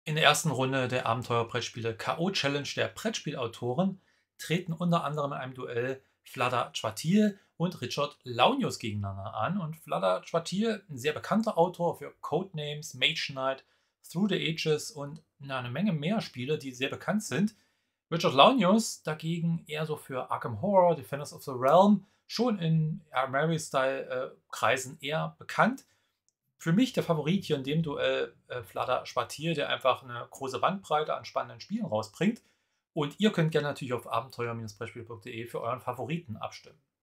Vlaada Chvátil oder Richard Launius? Brettspiel-Autoren K.O. Challenge Runde 1
Vlaada Chvátil oder Richard Launius?
Für wen stimmst du in der 1. Runde der Brettspiel-Autoren K.O. Challenge ab?